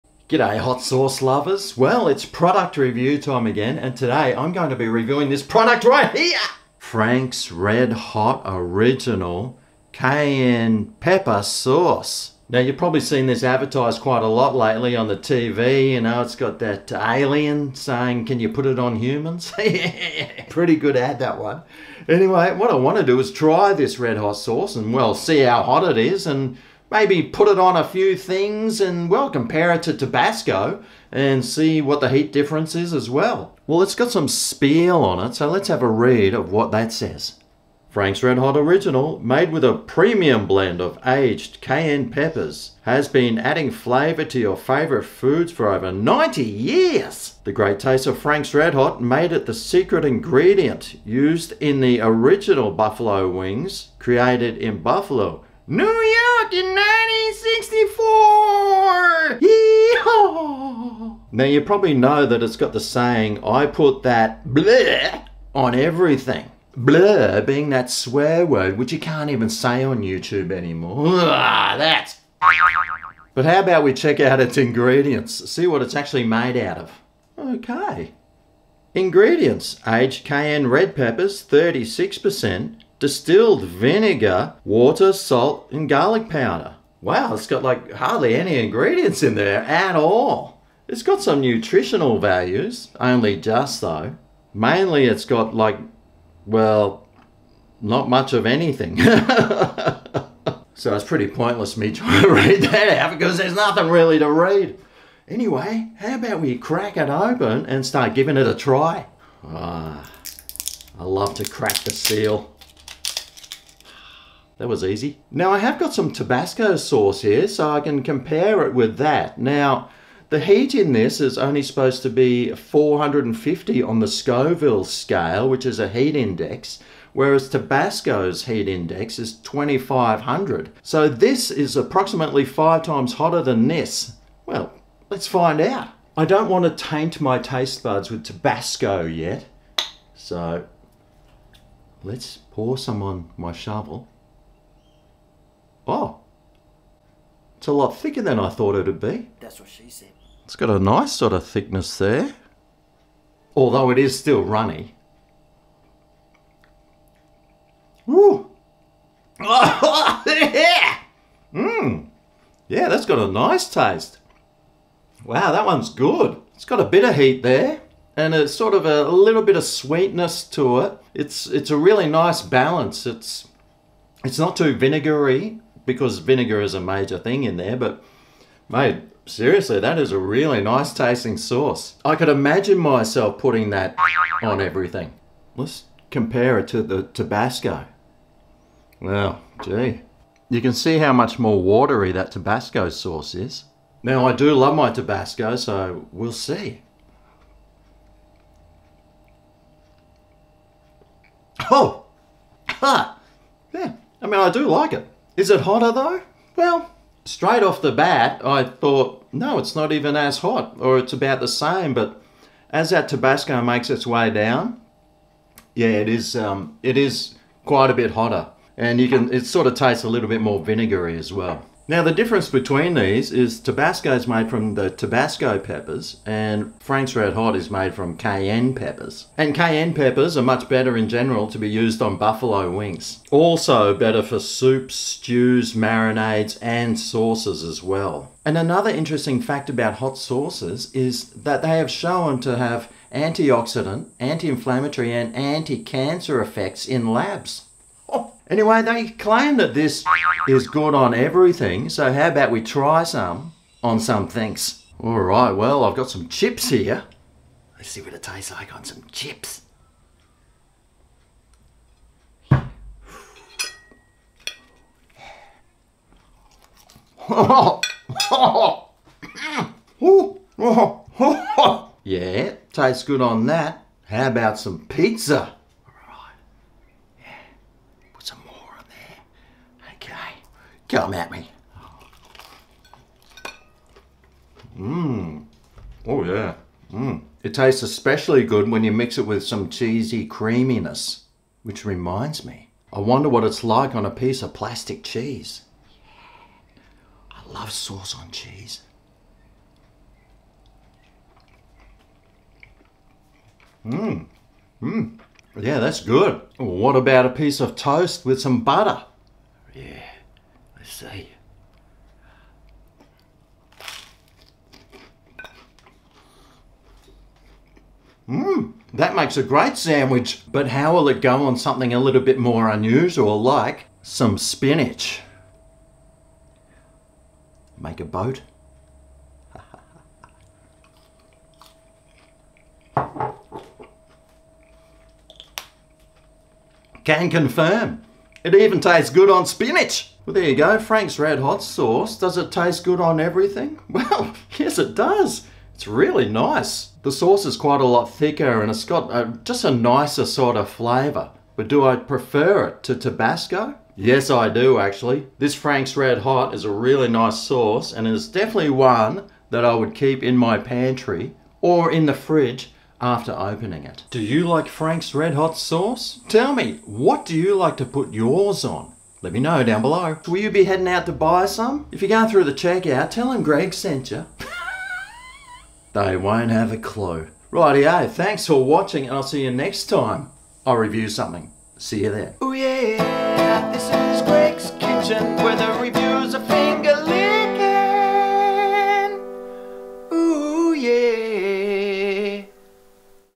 Is Franks Red Hot Sauce Better Than Tabasco?
It's product review time and today i'm trying out Franks Red Hot Sauce, lets give it a tast and see if this S**t is good on everything!

Help me buy food to review